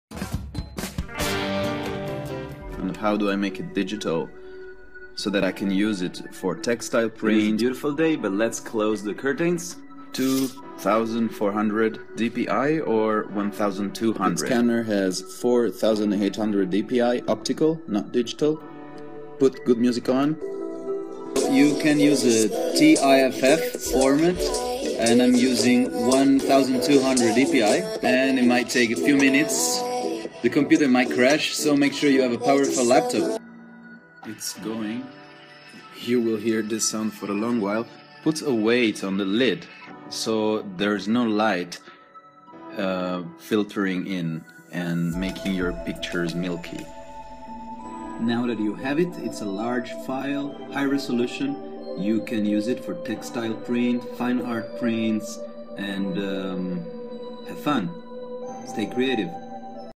How do I scan my art in HD - Colour Wave art
WOW! What?! High resolution scans of my art to print on my Society 6 t-shirts and for fine art prints, such as colorful posters.
Here is my process explained. Hope it helps.